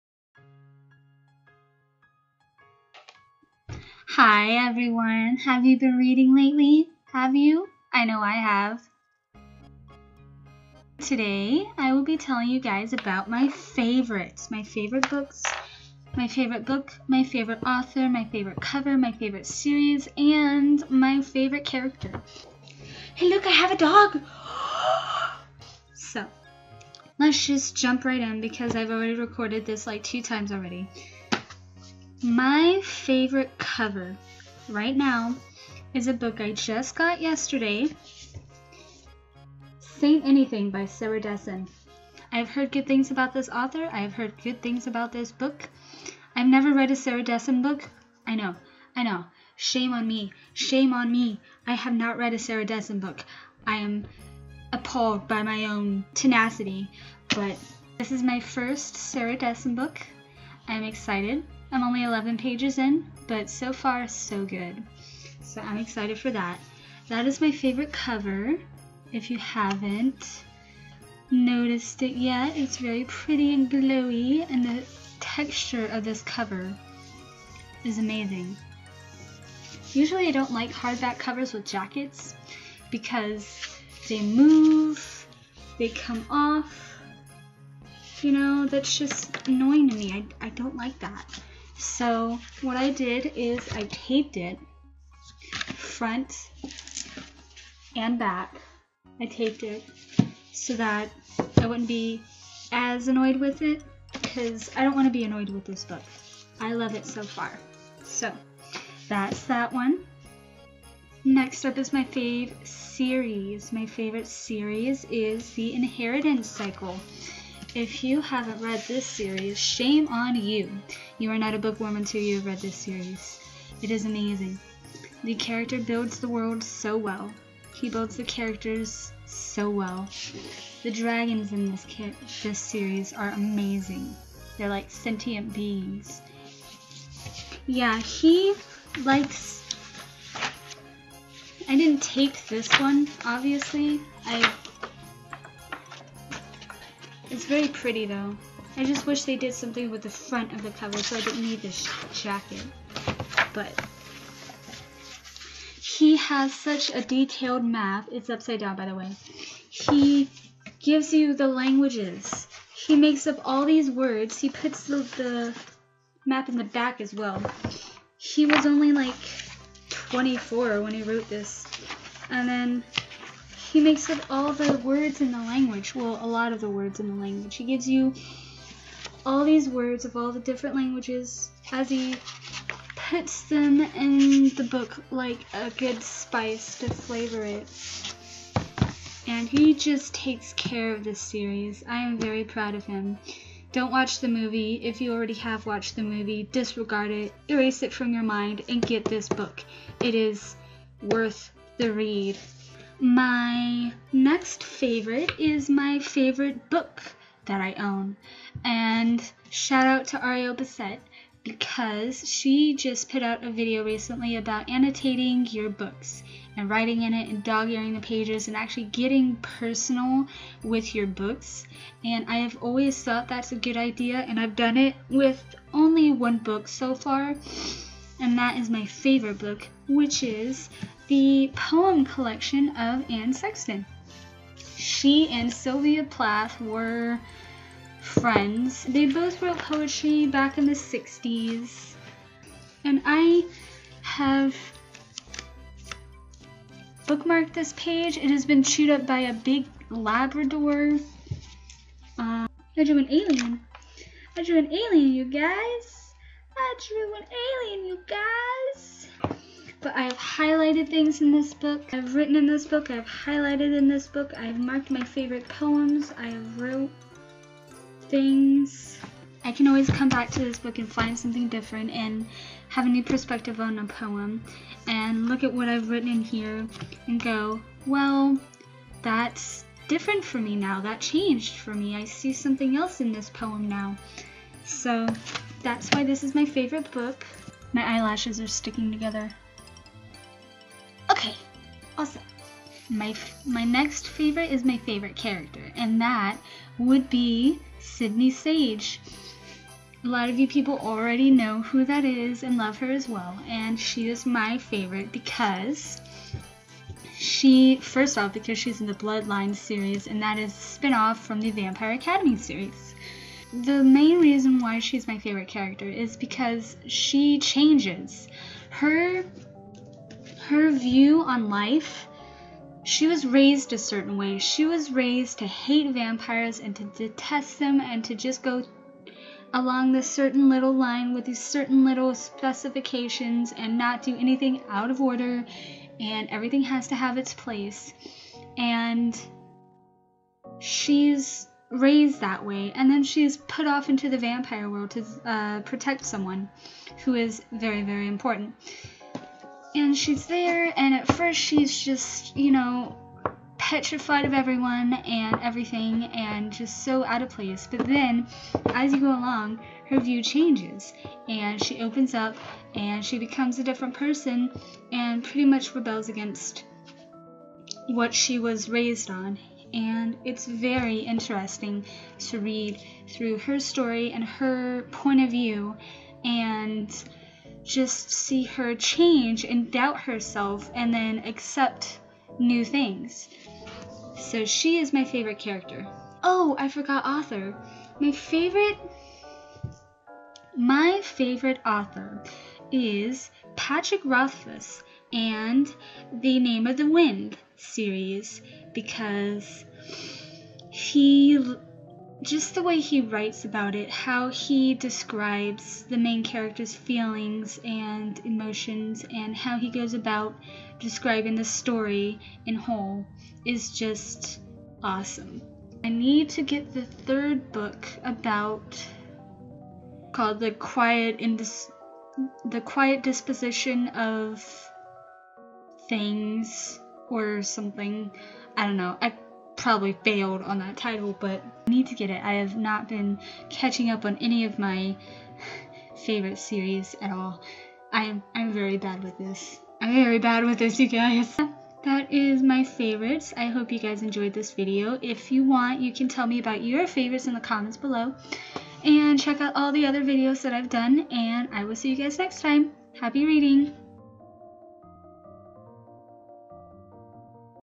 My Favorites
I tell you about my favorite cover, author, book, character, and series. what are your favorites?